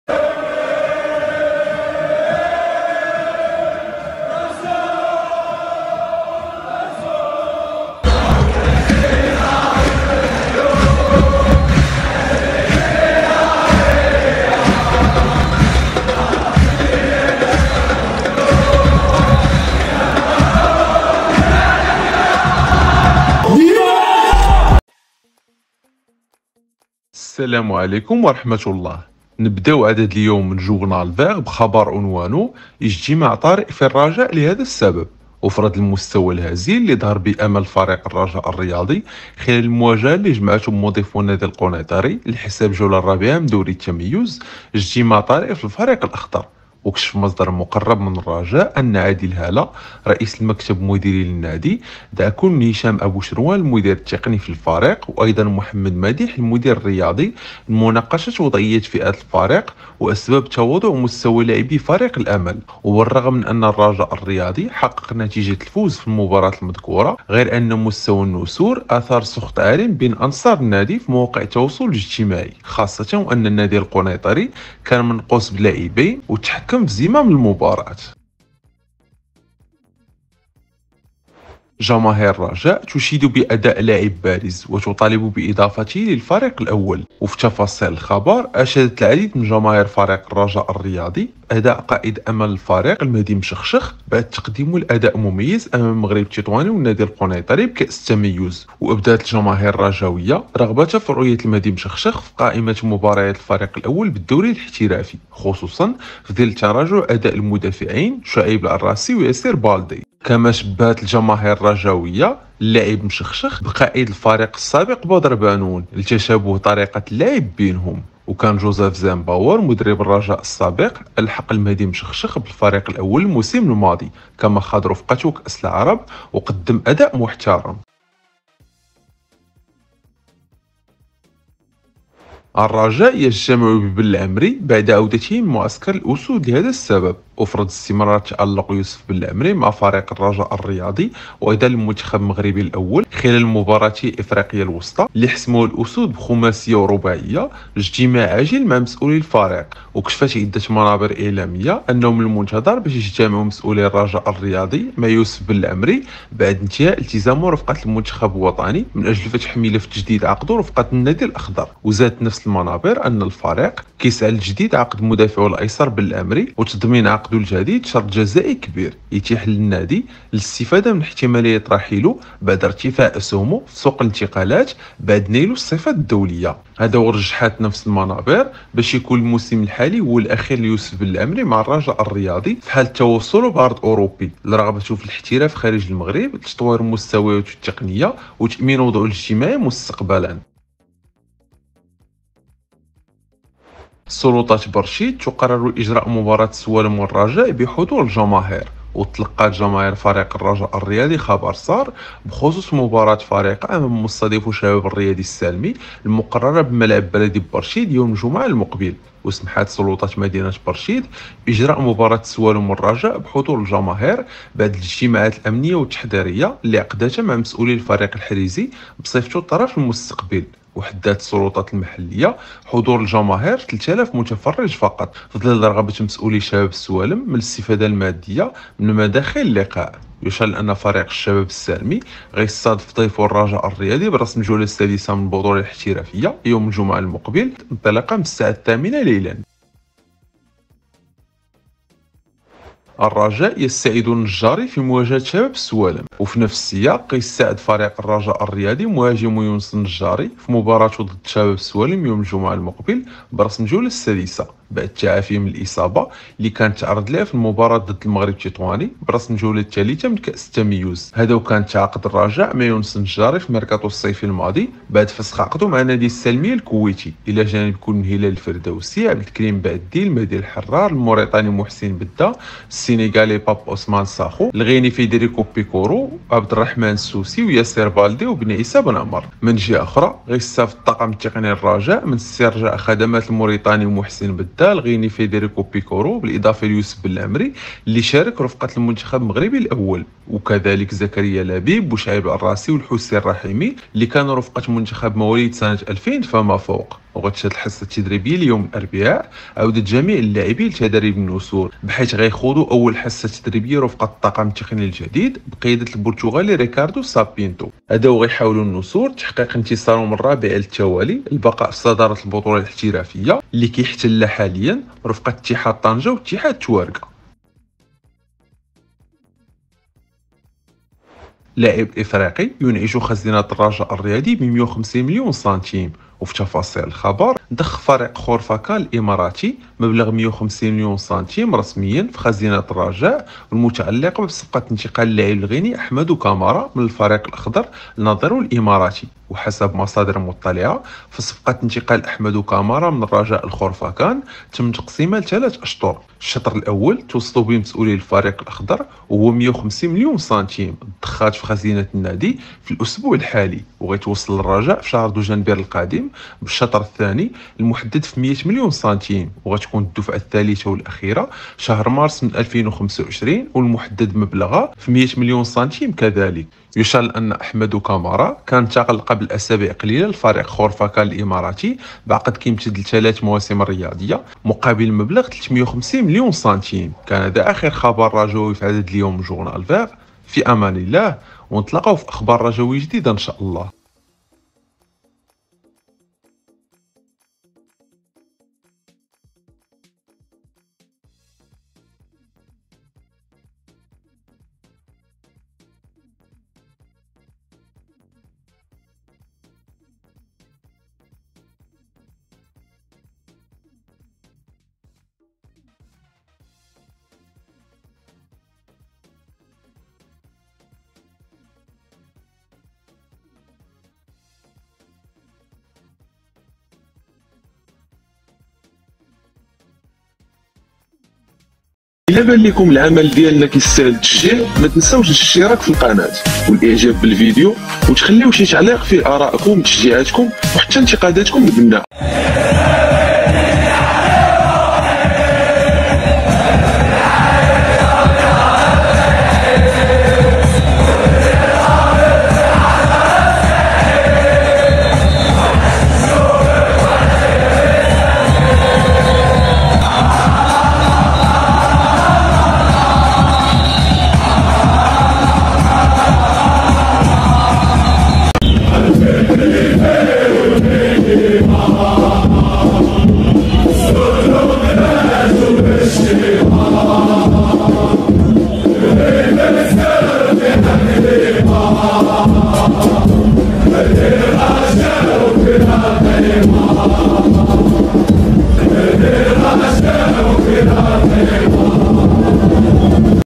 بانون الجديد في الرجاء | خطوة جديدة مع بلعمري | اجتماع عاجل بالرجاء | لاعب إفريقي ينعش خزينة الرجاء
لتشجيعنا على مواصلة العمل و خلق أفكار إبداعية رجاوية .. الإنتساب للقناة عبر هذا الرابط

جوغنالڤيغ  لهذا اليوم يتضمن آخر الأخبار الرجاوية من إعداد طاقم القناة و التي جاءت في الفيديو كاتالي :

ـ اجتماع طارئ في الرجاء الرياضي لهذا السبب ـ
ـ جماهير الرجاء تشيد بأداء مشخشخ وتطالب بإضافته للفريق الأول ـ
ـ الرجاء يجتمع بلعمري بعد عودته من معسكر الأسود ـ
ـ سلطات برشيد تقرر إجراء مباراة الرجاء بحضور الجماهير ـ
ـ الرجاء يستعيد النجاري في مواجهة شباب السوالم ـ
ـ لاعب إفريقي ينعش خزينة الرجاء بـ 150 مليون سنتيم ـ

لا تنسوا الإشتراك فالقناة و تفعيل الجرس و ترك تعليقاتكم على الفيديو لكي يصلكم الجديد أولا بأول.

فرجة ممتعة.